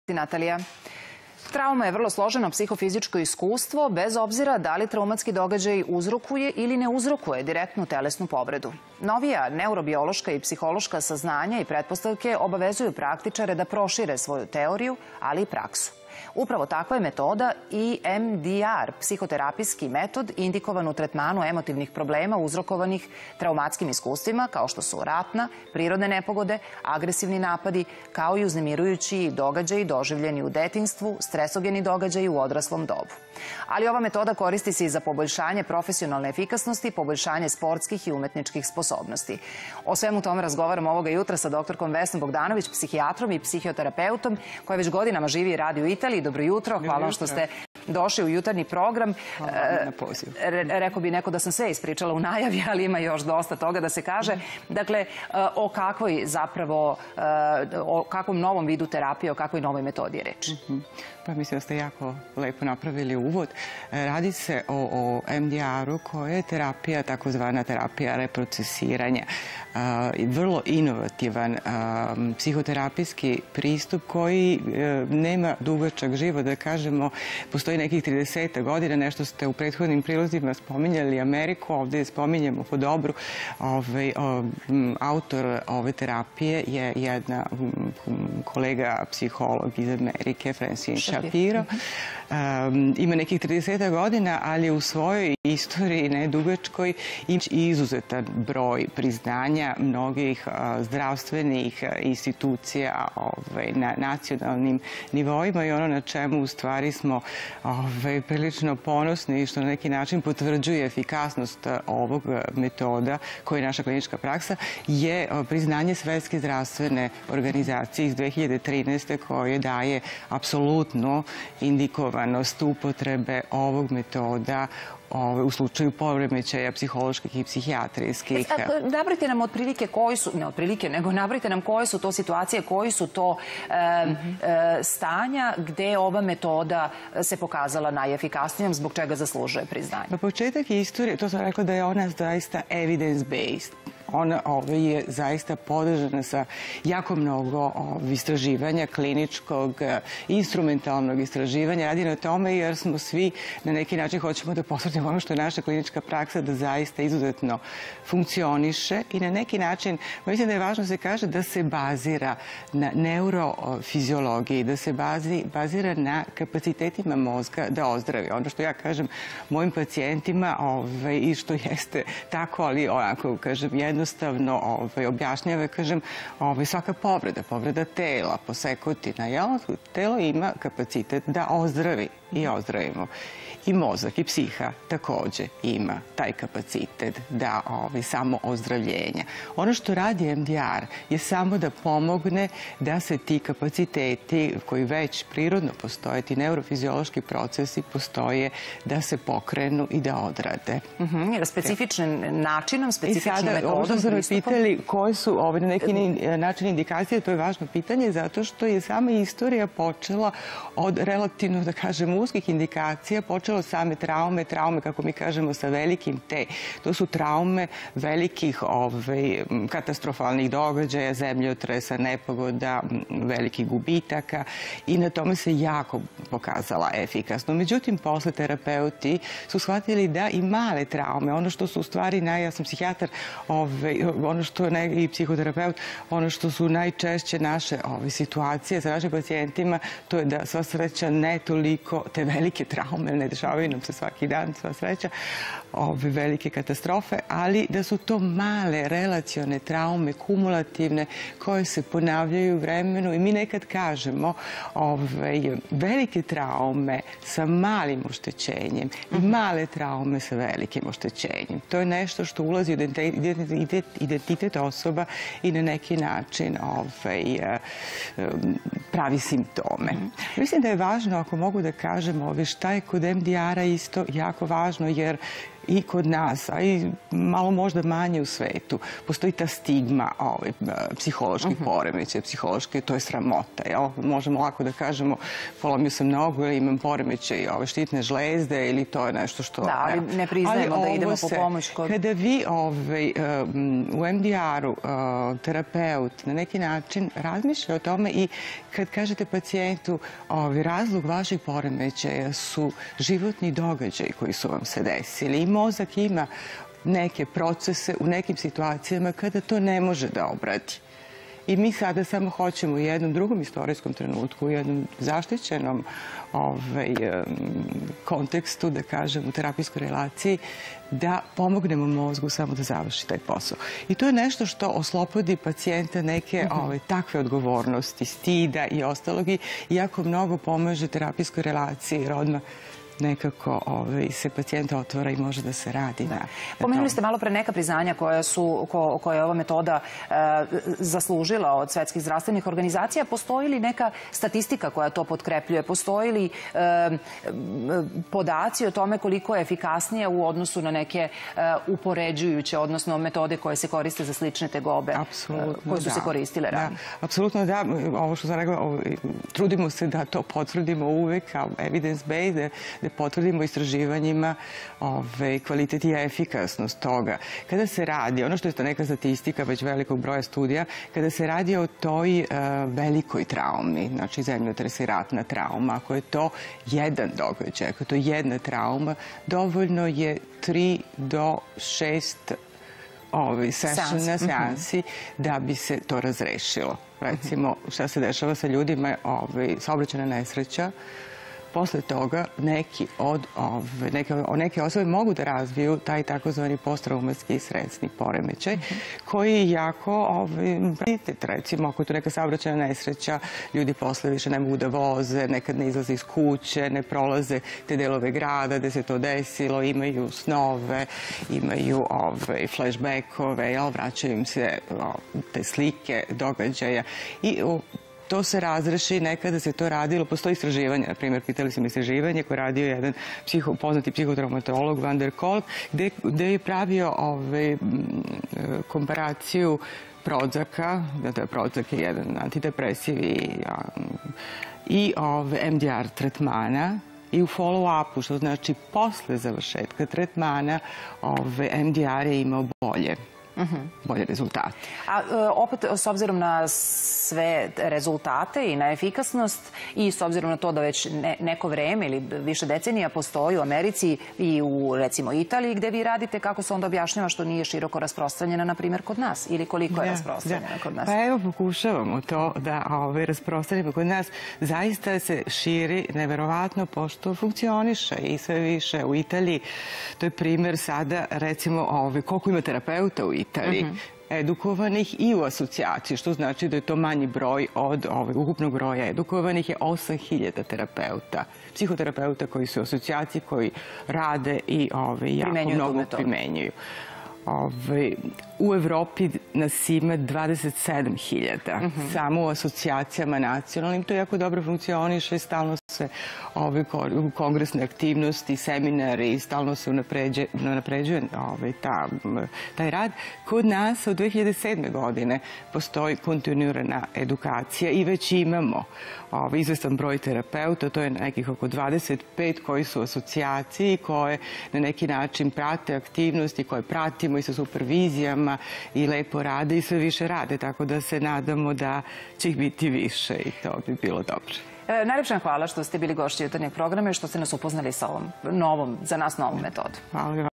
Terapija traume - EMDR, psihoterapijski metod
Trauma je vrlo složeno psihofizičko iskustvo, bez obzira da li traumatski događaj uzrokuje ili ne uzrokuje direktnu telesnu povredu. Novija neurobiološka i psihološka saznanja i pretpostavke obavezuju praktičare da prošire svoju teoriju, ali i praksu. Upravo takva je metoda - EMDR, psihoterapijski metod indikovan u tretmanu emotivnih problema uzrokavanih traumatskim iskustvima kao što su ratna iskustva, prirodne nepogode, agresivni napadi, kao i uznemirujući dogadjaji doživljeni u detinjstvu, stresogeni dogadjaji u odraslom dobu. Ali, ova metoda koristi se i za poboljšanje profesionalne efikasnosti, poboljšanje sportskih i umetničkih sposobnosti. O svemu tome razgovarali smo sa Dr Vesnom Bogdanović, psihitarom i psihoterapeutom, koja već godinama živi i radi u Italiji.